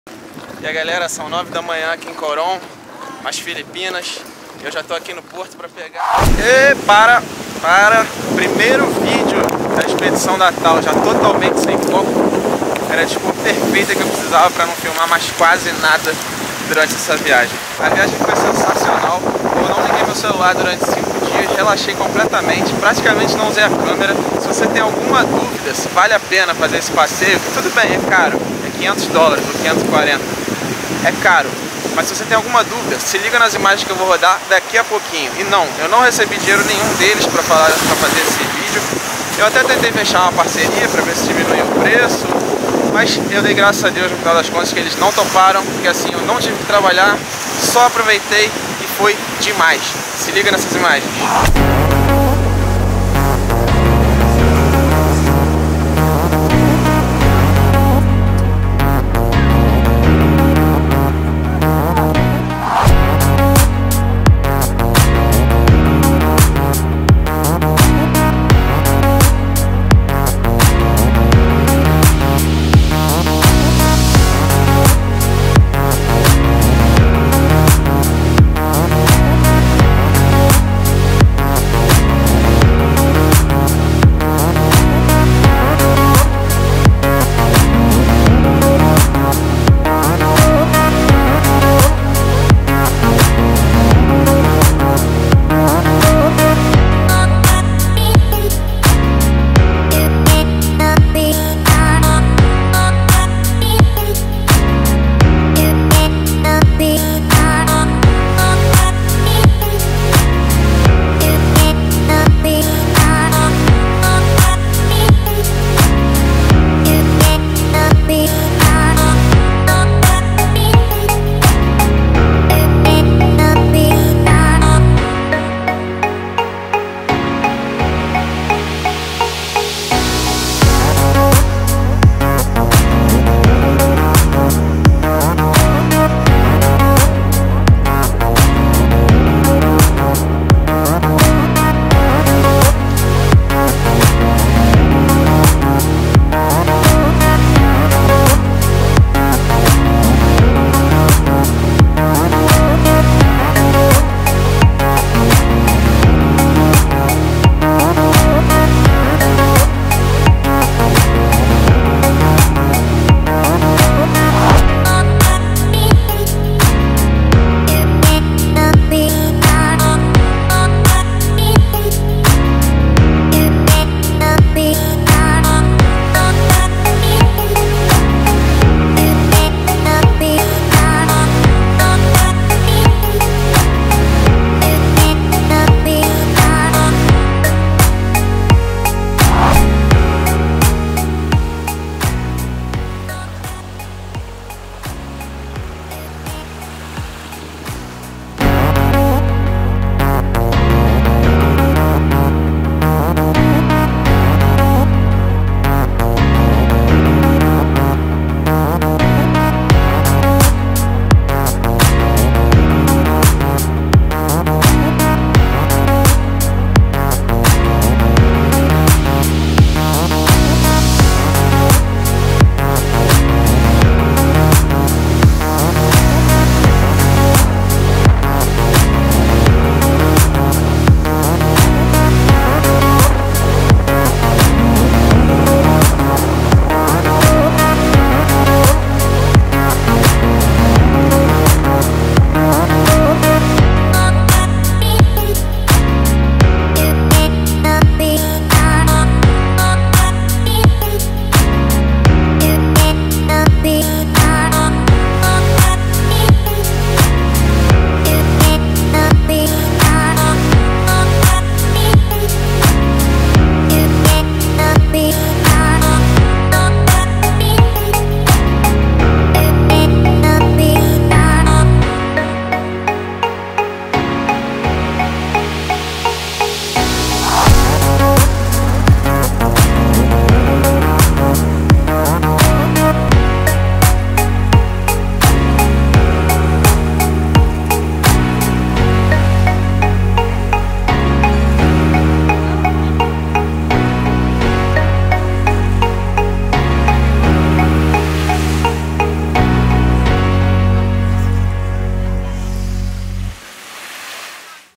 A MELHOR COISA QUE FIZ - Expedição Tao Philippines
O Passeio de barco da Tao Philippines.

Numa volta ao mundo de 1 ano, é difícil escolher os momentos mais especiais, mas seguramente esses 5 dias totalmente desligado de tudo no mundo no barco da Tao Philippines estarão na minha memória pra sempre, e talvez como o destaque número 1 da minha viagem.

É mais que um tour, é uma experiência única em que os 5 dias parecem quase um mês de tanta intensidade, ou somente algumas horas, porque tudo que é muito bom passa muito rápido!

Até tem sinal de celular em algumas ilhas, mas eu preferi desligar o meu durante os 5 dias e só o usar como lanterna pra tomar o banho de baldinho de noite.

Os guias, a comida deliciosa, os lugares mais bonitos que já vi na vida, a paz de espírito, e a sorte que eu dei com o meu grupo fizeram desses 5 dias o melhor momento de toda essa viagem maravilhosa!

Quase não filmei nada porque quis curtir ao máximo o que eu estava vivendo. Foi demais! Se querem uma dica, a dica é: Nem pensem! Marquem logo esse tour na sua trip pras Filipinas!

56 países visitados! Vídeos das minhas viagens ao redor do mundo!